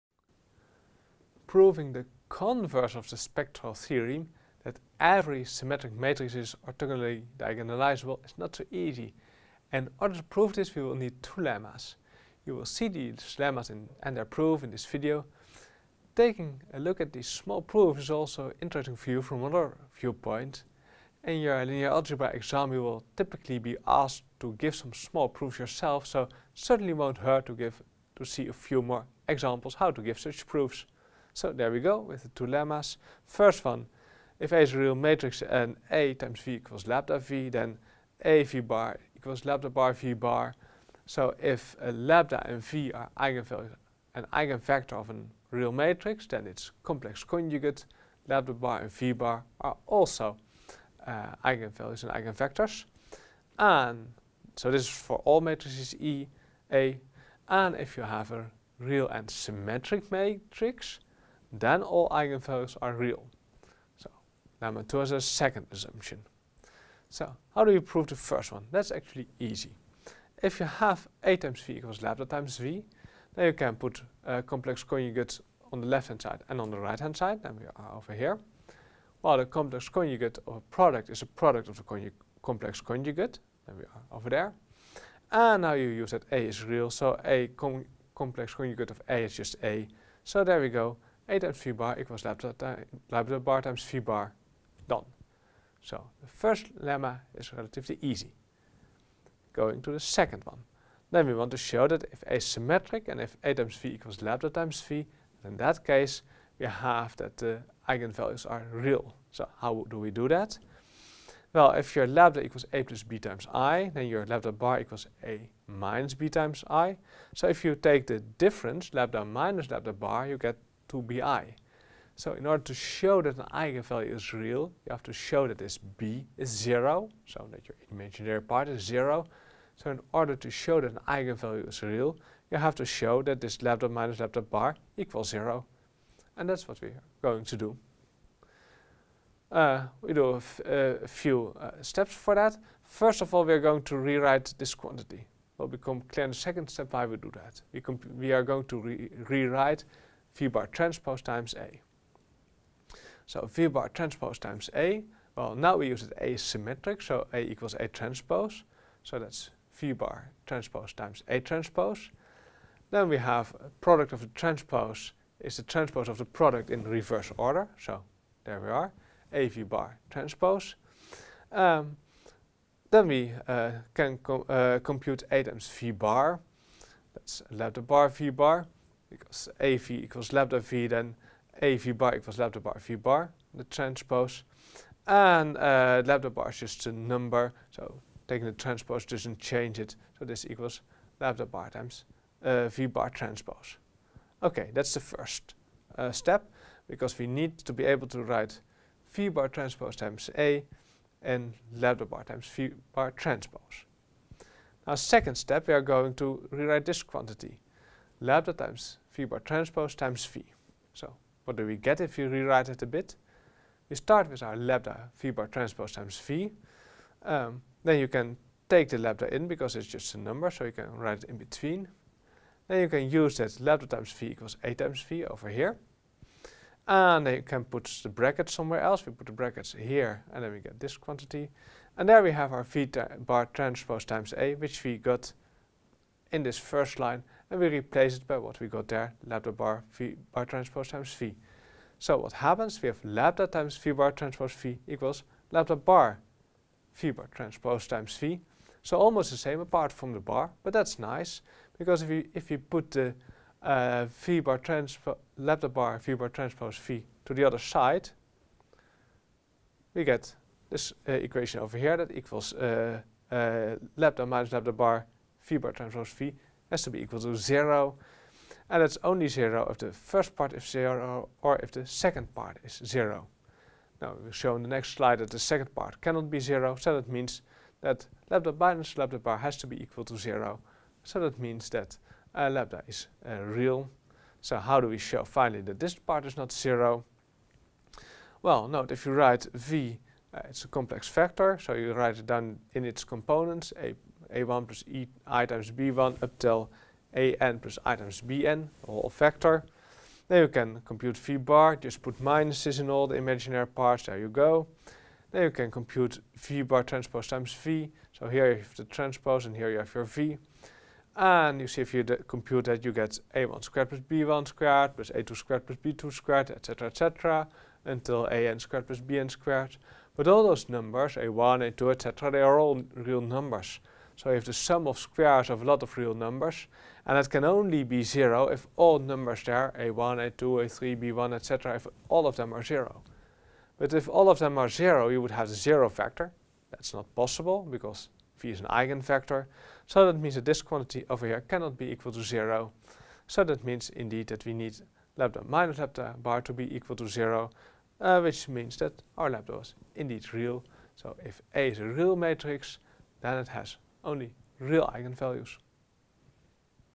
The spectral theorem - preliminaries
In this preliminary video we show that a symmetric matrix has only real eigenvalues.